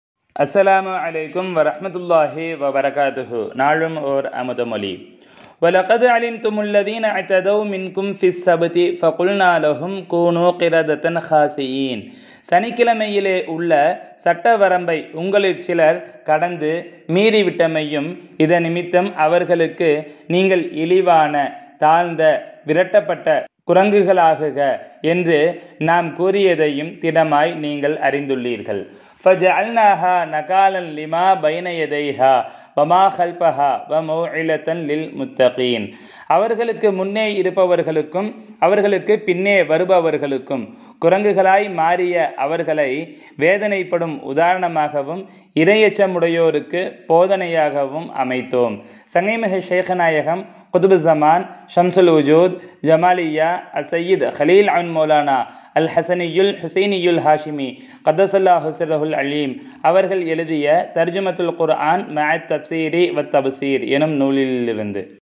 இறை தண்டனை பெற்றோரும் நமக்கான படிப்பினை-Tamil Tafseer|குர்ஆன் தஃப்ஸீர்| Tamil Quran | AlQuran Tamil
** தர்ஜூமதுல் குர்ஆன் மஅத் தஃப்ஸீரி வத்  தஃப்ஸீர் ** [ முதல் ஜூஸ்வு ] - JSKAAH MOULANA

குர்ஆன் தஃப்ஸீர் -  Quran Tafseer

தலைப்பு: இறை தண்டனை பெற்றோரும் நமக்கான படிப்பினை

Tamil Tafseer || குர்ஆன் தஃப்ஸீர் || Tamil Quran || தமிழ் குர்ஆன் || Al Quran Tamil